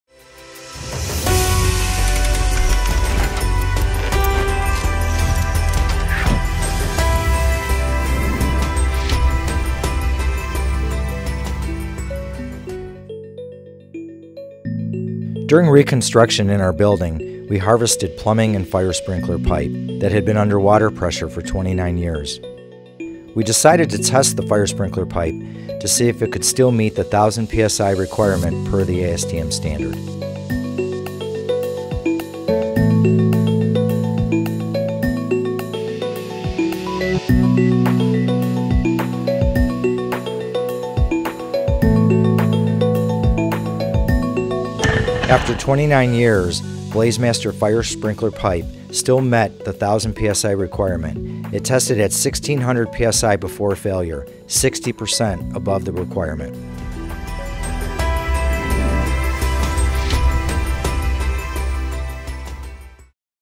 Harvested BlazeMaster CPVC Exceeds Requirements
BlazeMaster Fire Protection Systems that were under pressure for 29 years not only met the ASTM standard for 1,000 PSI, but tested 60% above the requirement before failure.